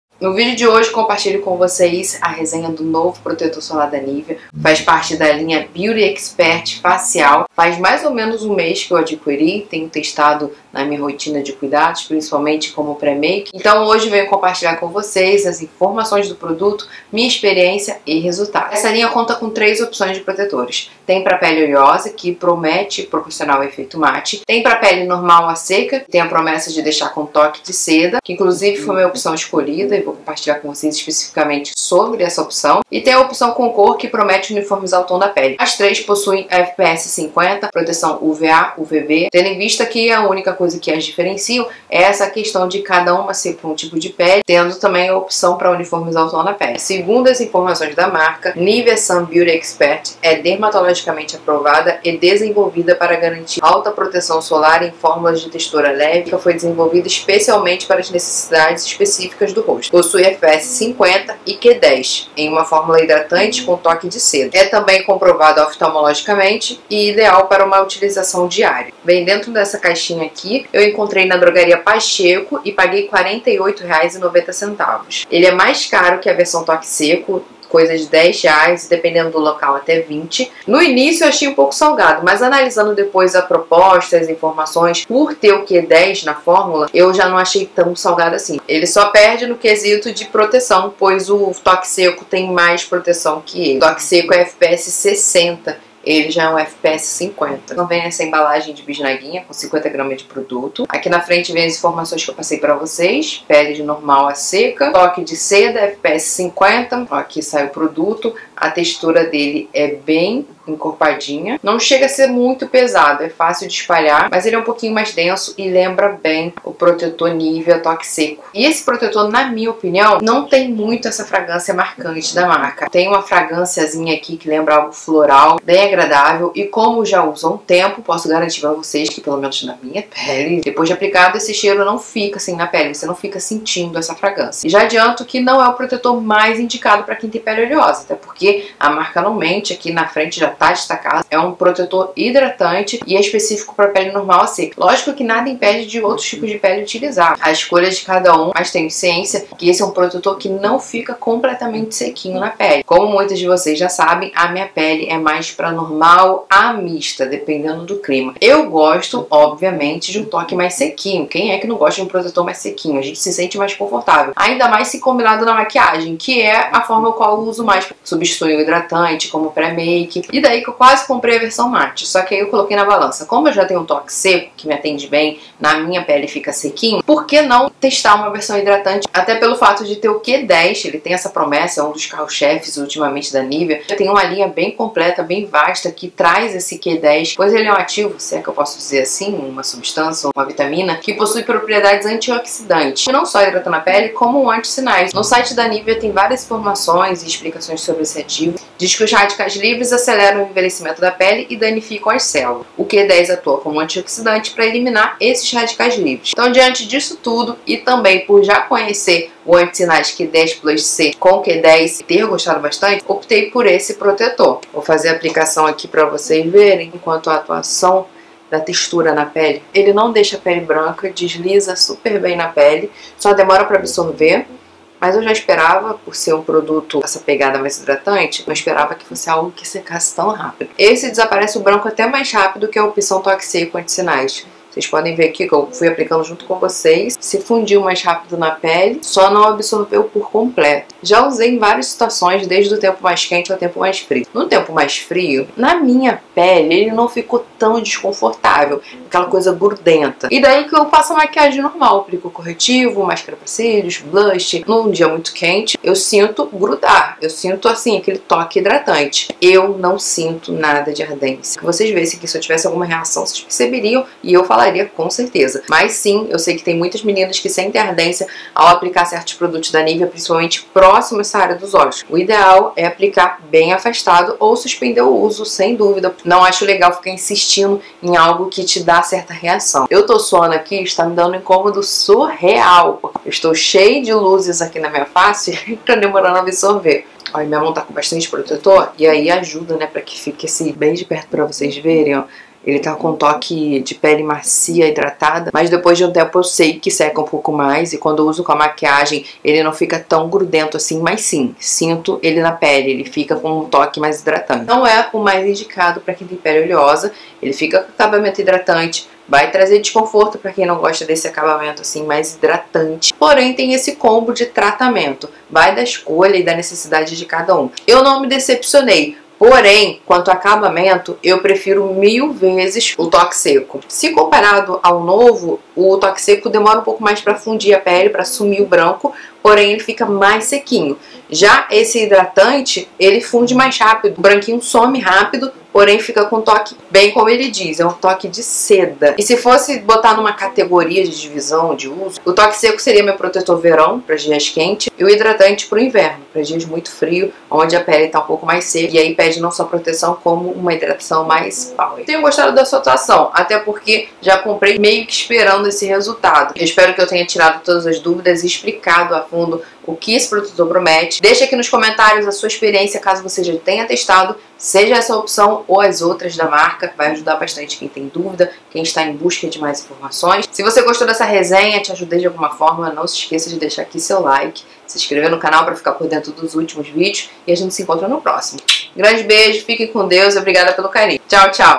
NOVO PROTETOR NIVEA SUN | HIDRATANTE COM Q 10
TESTEI O NOVO PROTETOR DA NIVEA, PROTETOR SOLAR HIDRATANTE COM Q10, E NESTE VÍDEO COMPARTILHO TODOS OS DETALHES E A MINHA EXPERIÊNCIA . ;)

▷ OUTROS VÍDEOS